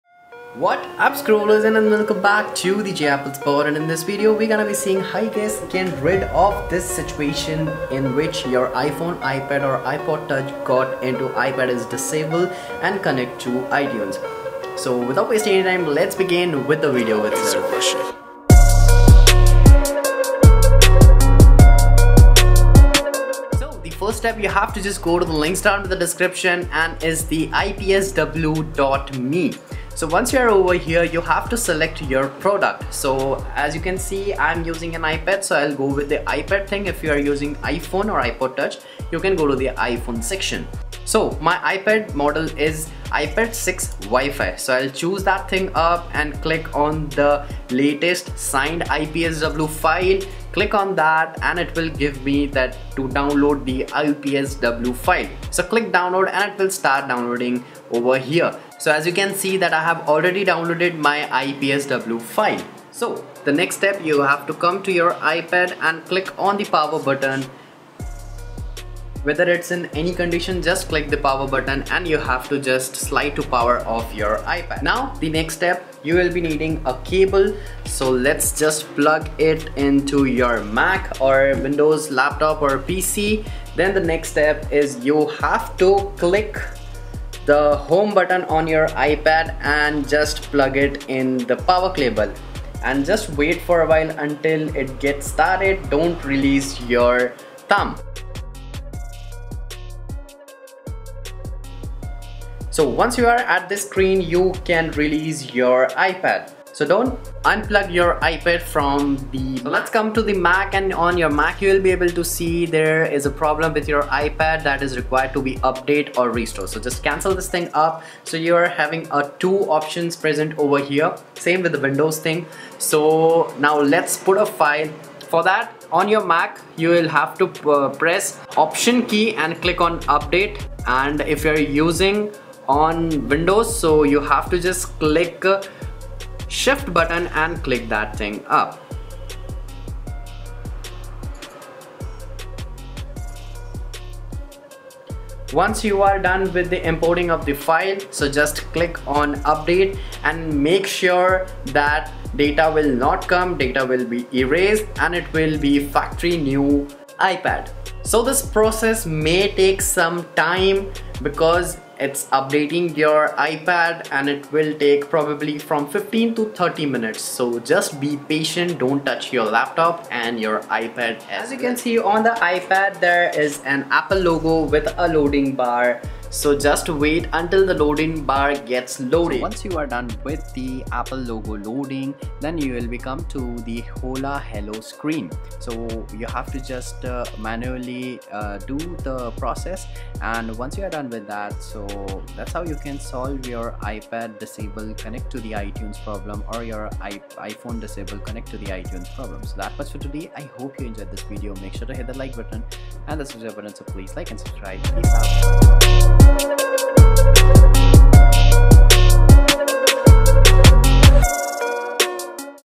How to Fix iPad is Disable Connect to iTunes in 2021
In this video i have show some easy steps if you iPad or iPhone is Disable or Forgotten Password.

Have a look Amazing Content Creator 😍